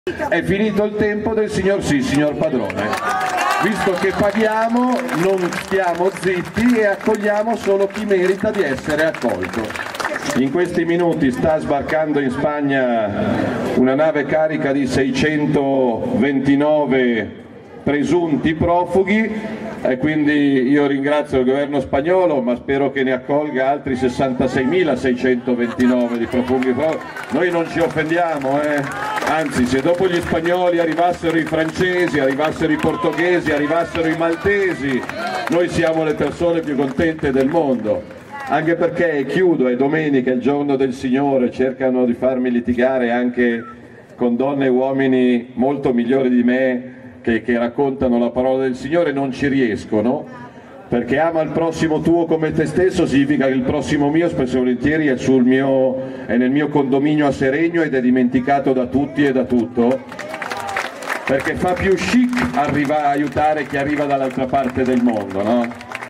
Migranti, Salvini: "In Spagna oggi 629 ma se ne accoglie altri non ci offendiamo"
POLITICA (Seregno). "È finito il tempo del signorsì padroni. Accogliamo solo chi merita di essere accolto. In questi minuti sbarca in Spagna una nave carica di 629 presunti profughi: ringrazio il Governo spagnolo ma spero ne accolga altri 629, noi non ci offendiamo!
Anzi se arrivassero anche francesi profughesi e maltesi saremmo i piu contenti del mondo. Ama il prossimo tuo, sì, ma anche il vicino di condominio", così il Ministro degli Interni Matteo Salvini questa mattina da Seregno dove si trovava per la campagna elettorale del comune. (Livia Caivano/alaNEWS)